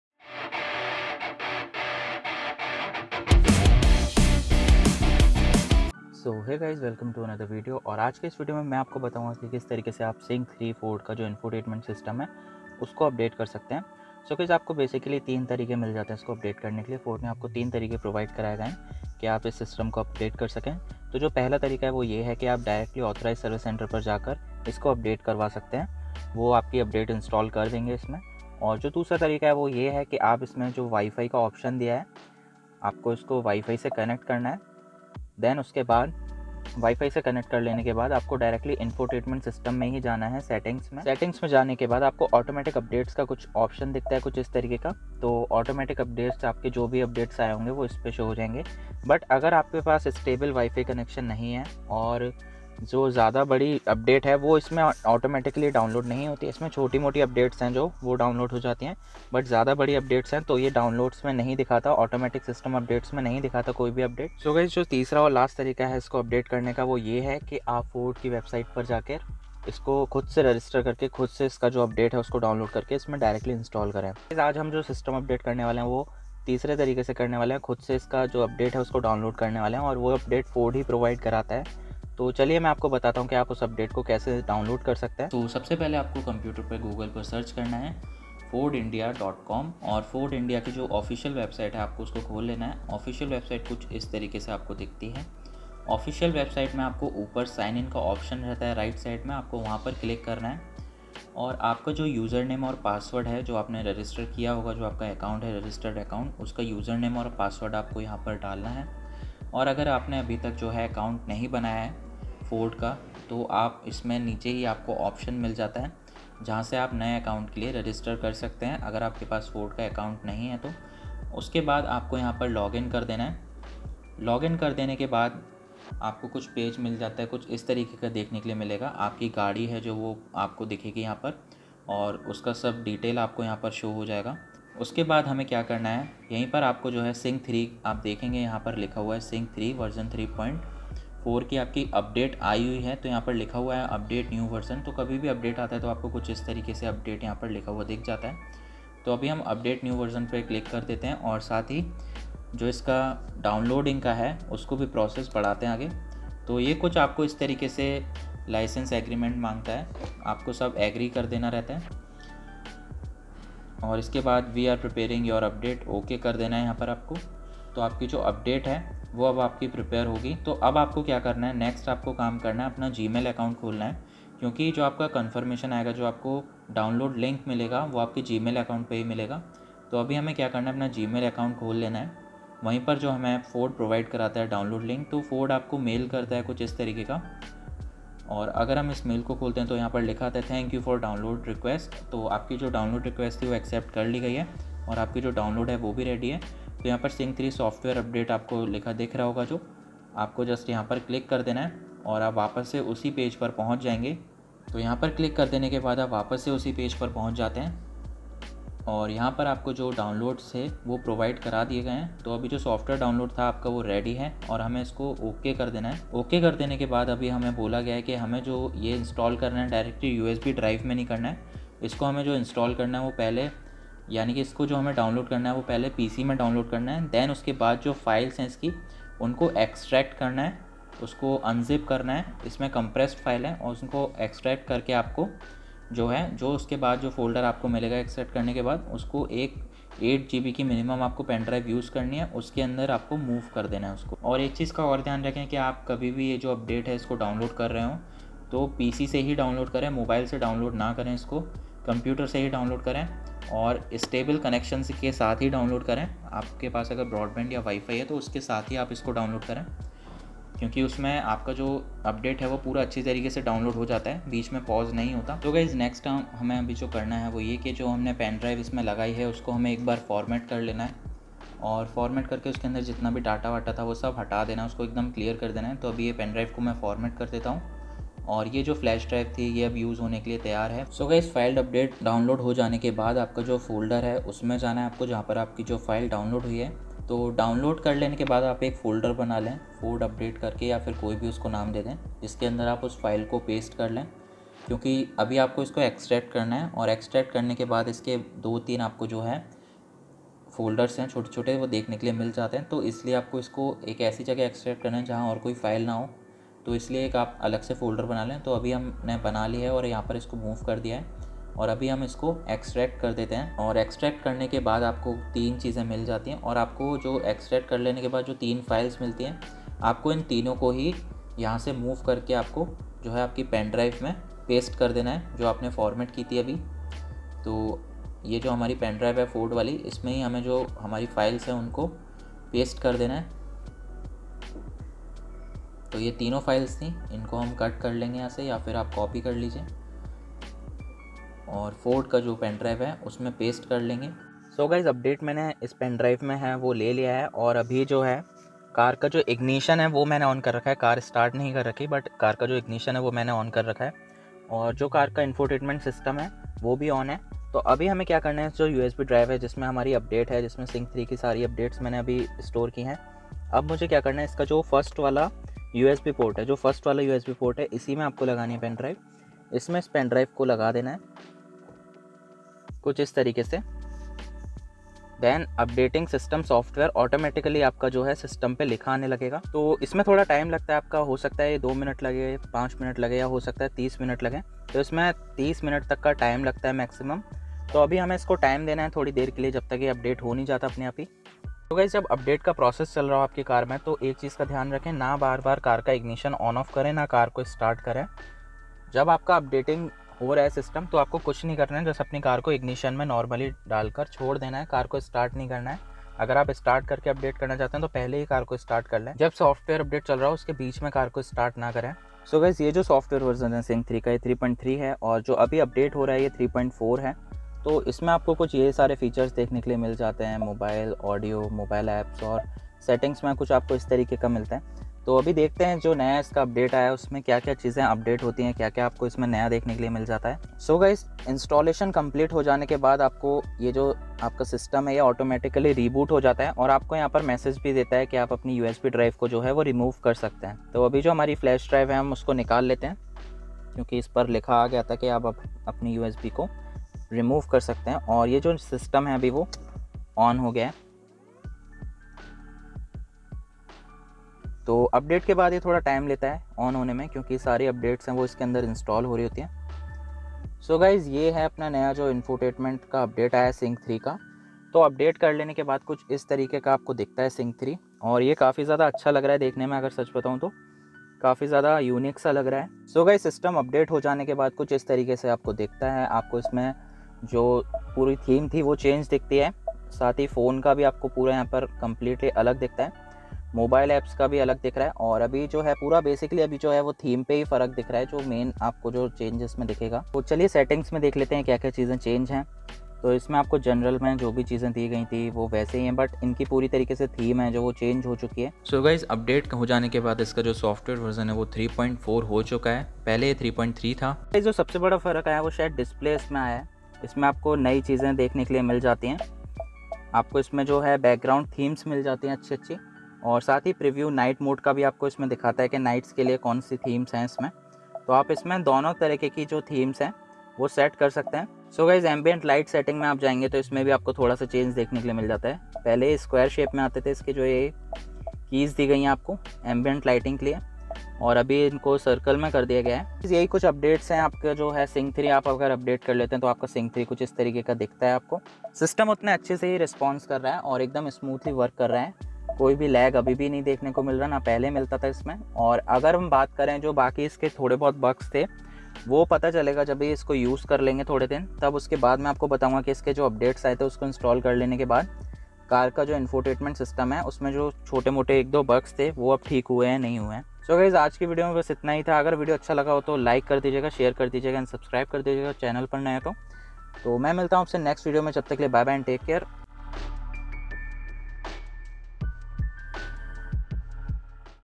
Sync-3 Update Ford|How To Update Sync 3 V3.4|Easy Steps|Hindi|Ford Ecosport S
Hello dosto is video mai bataya gya hai kese aap sync 3 system ko update kr skte hai.

Note-This is not promotional video and not promoted to anyone. Please don't spend money before investigation. We are not liable for the same. It's only for education and information